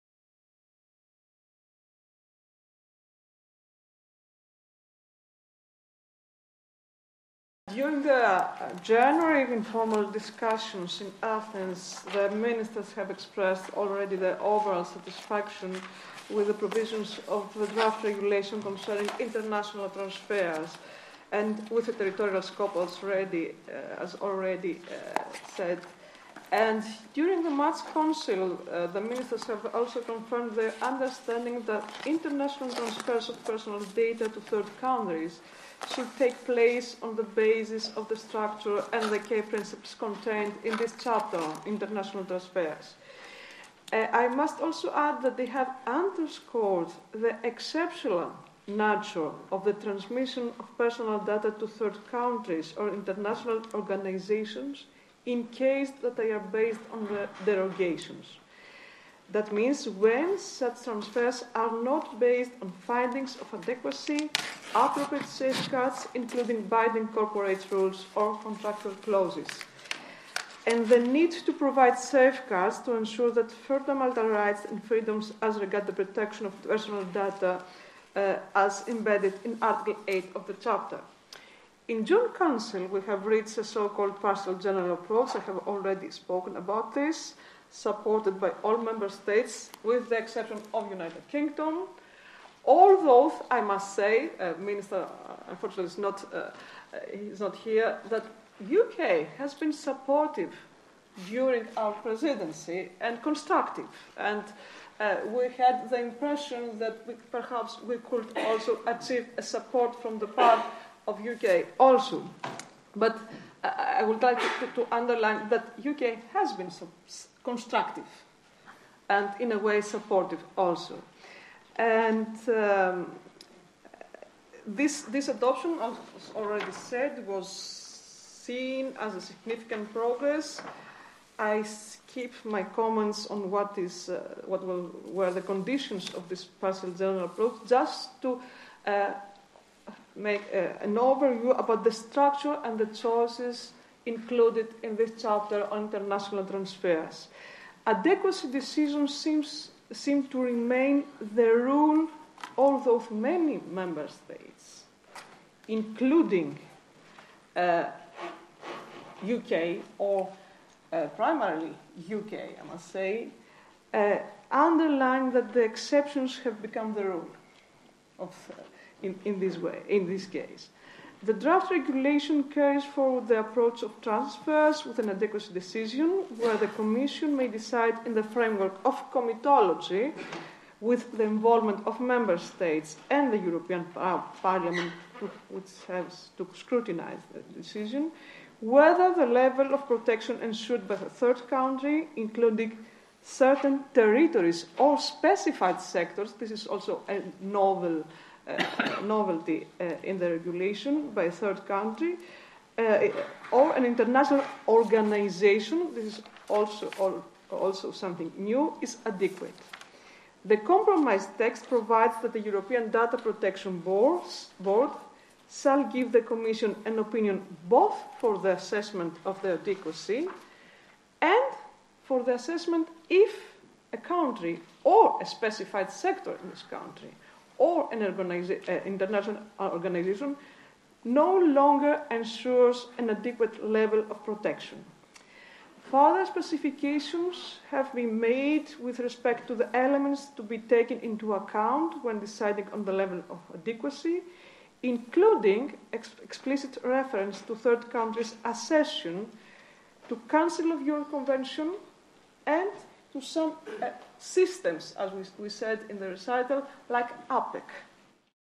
US Safe Harbor: Surveying the foundations and other aspects of international transfers
Speaker: Lilian Mitrou, Asssociate Professor, University of the Aegean, Greece, Chair DAPIX Council of Justice
Ministers Committee, Greek Presidency, January - June 2014
Recorded at the Privacy Laws & Business 27th Annual International Conference
New Horizons - New Risks
30 June - 2 July 2014, Cambridge, UK

• How the Safe Harbor works
• Safe Harbor as a driver of stronger privacy policies for transfers of personal data from the European Economic Area to the US, backed by FTC sanctions
• Steps to strengthening the Safe Harbor foundations
• The European Commission’s requests for strengthening the Safe Harbor system
• The strengths and weaknesses of the EU’s Binding Corporate Rules
• The strengths and weaknesses of EU model contracts
• The strengths and weaknesses of APEC’s Cross-Border Privacy Rules
• Searching for a path towards legally sound international transfers of personal data